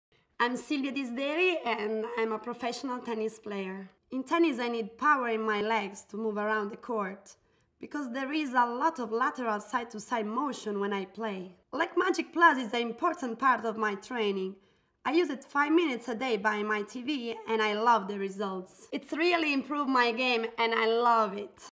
Silvia Disderi Leg Magic Plus workout tv commercial
Silvia Disderi Italian wta tennis player presents Leg Magic Plus on tv commercial (Sep.2015)